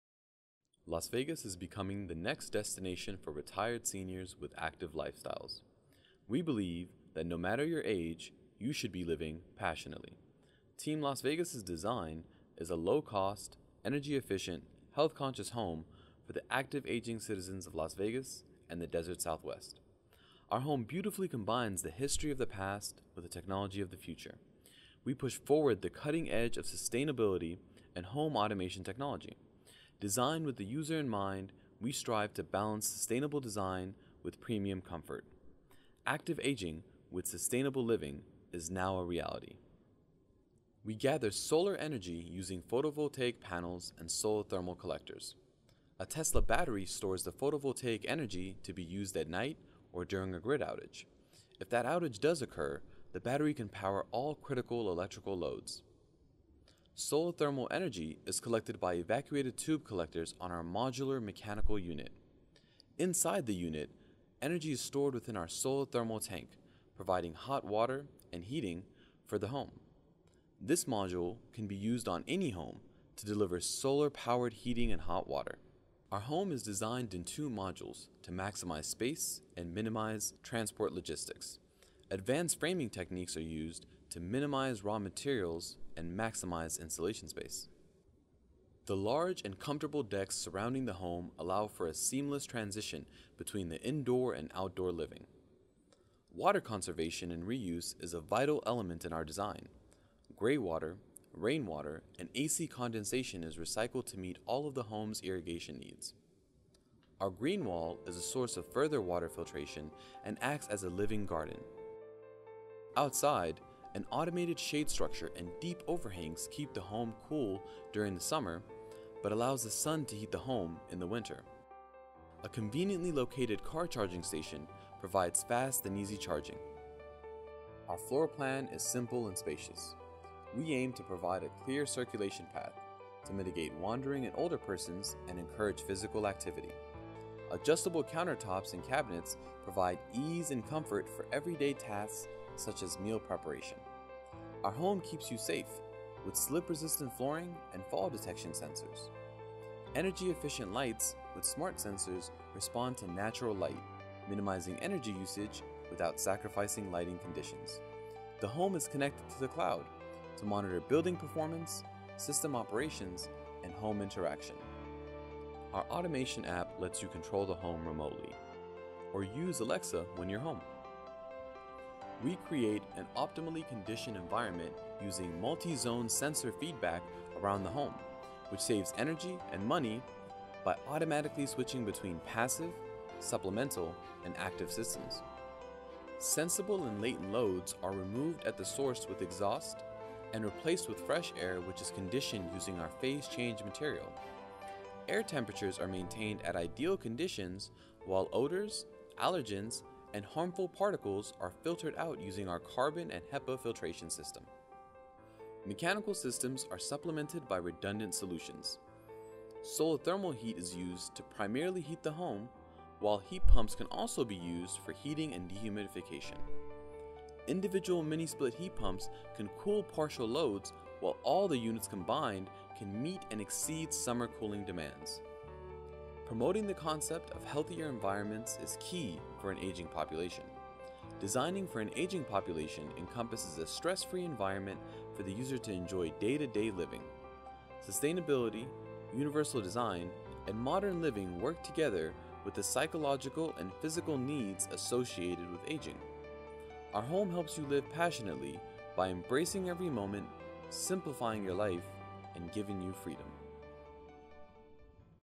Las Vegas Digital Project Representation - Solar Decathlon 2017
Video and/or computer-animated walkthrough of the design from the University of Nevada, Las Vegas team, submitted as part of the Design Development deliverable, representing approximately 90% complete design.

The U.S. Department of Energy Solar Decathlon competition challenges collegiate teams to design and build full-size, energy-efficient, solar-powered houses. Free to visitors, October 5 – 9 and 12 – 15, 2017, at the 61st & Peña Station, Denver, CO, the event features a sustainability expo, education events, and a community festival.